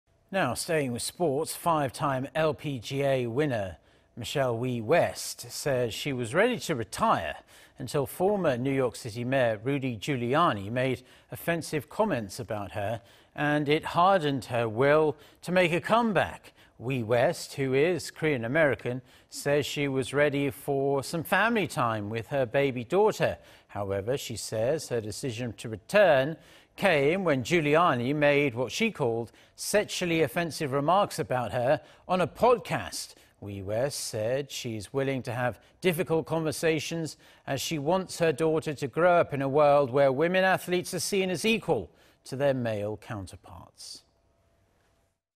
5-time LPGA champ Michelle Wie West says Giuliani's offensive remarks prompted come back
미셸 위 LPGA 복귀 이유는 줄리아니 성희롱 발언

Five-time LPGA winner Michelle Wie West says she was ready to retire until former New York City mayor Rudy Giuliani made offensive comments about her, and it hardened her will to comeback.
Wie West, who is Korean-American, says she was ready for family time with her baby daughter.
However, she says her decision to return came when Giuliani made what she called sexually offensive remarks about her on a podcast.
Wie West said she's willing to have difficult conversations as she wants her daughter to grow up in a world where women athletes are seen as equal to their male counterparts.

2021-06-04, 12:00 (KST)